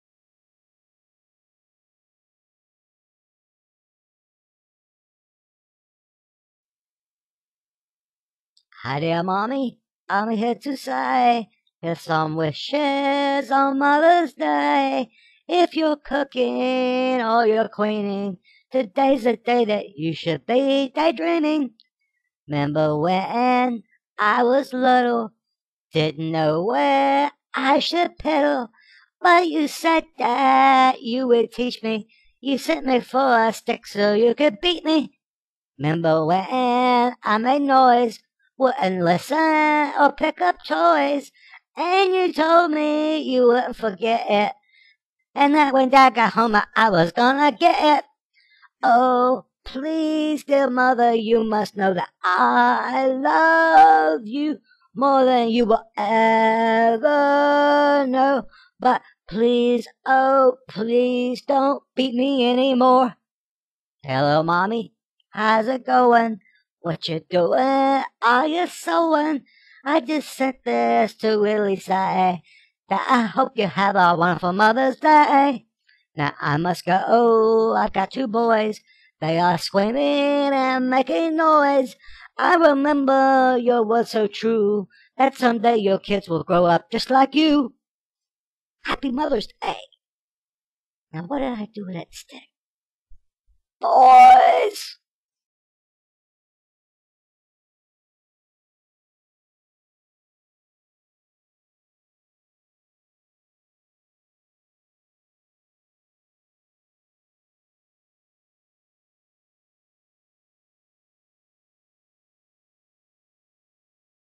Mother's Day Song
This cute little funny dog sings to your mom in this clip, telling her what every mother needs to hear.